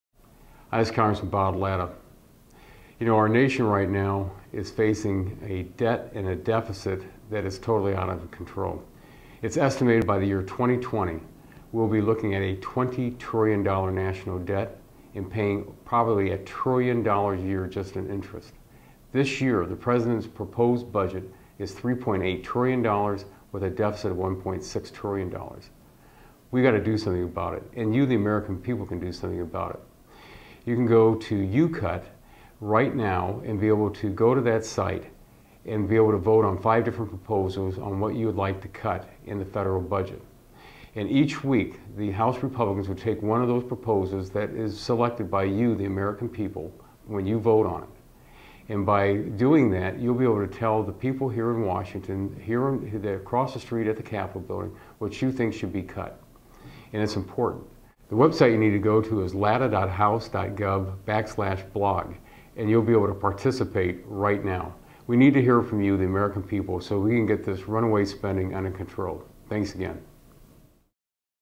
YouCut: Make Your Voice Heard!
Congressman Bob Latta (R-OH) encourages participation in YouCut: Make Your Voice Heard.